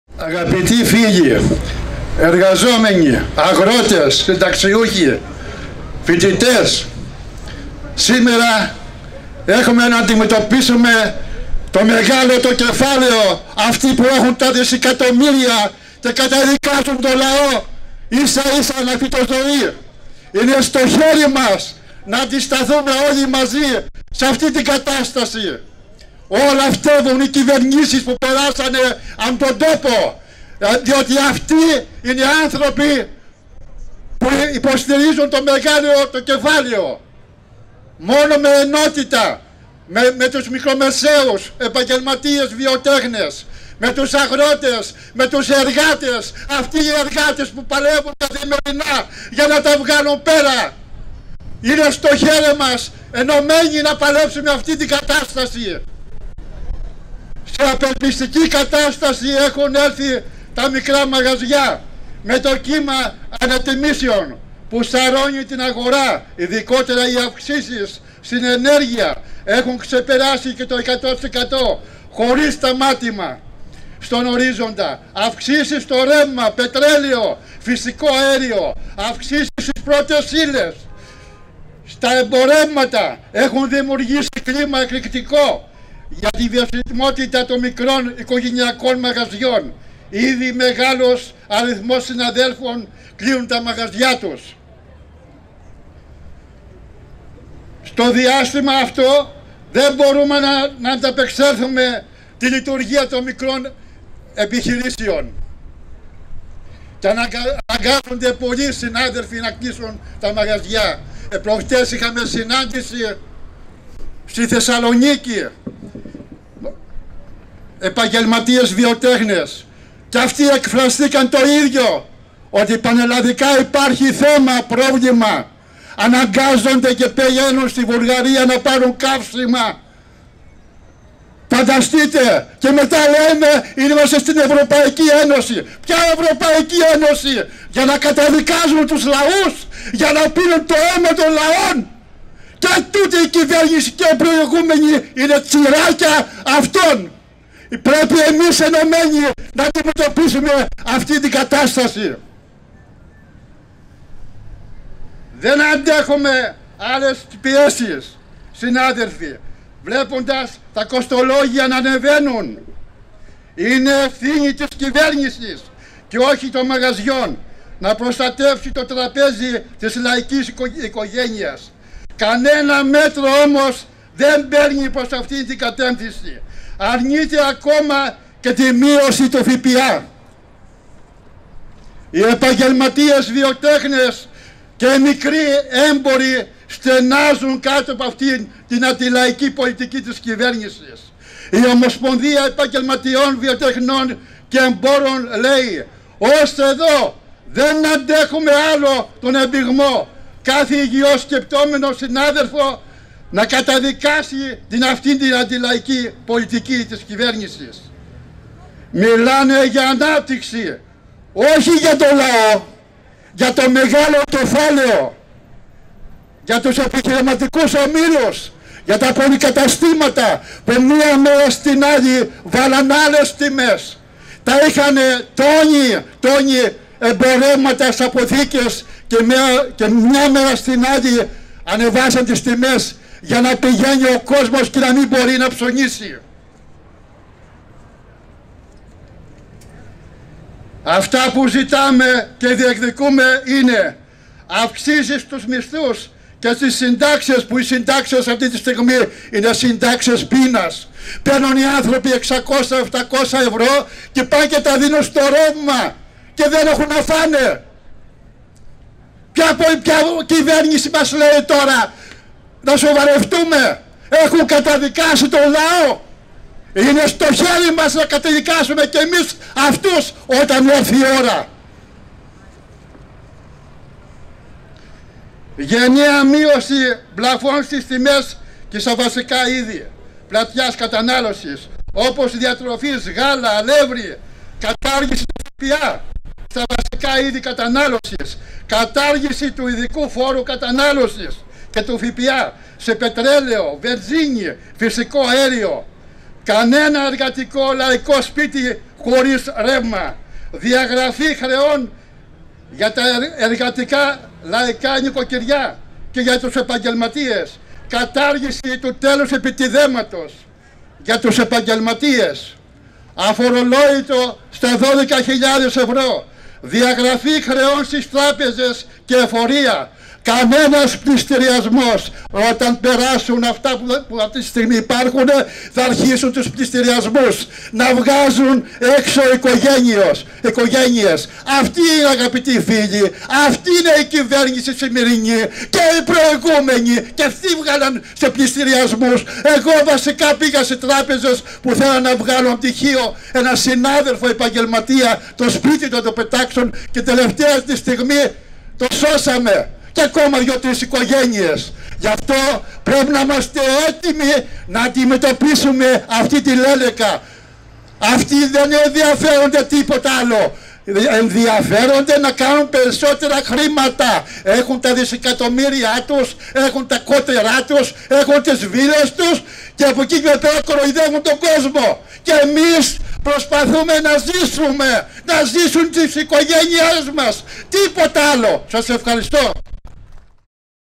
Δήλωση του Νίκου Μπουλμπούλη, Προέδρου Ομοσπονδίας Επαγγελματιών, Βιοτεχνών και Εμπόρων Λέσβου
Απεργιακή κινητοποίηση 6 Απριλίου στην πλατεία Σαπφούς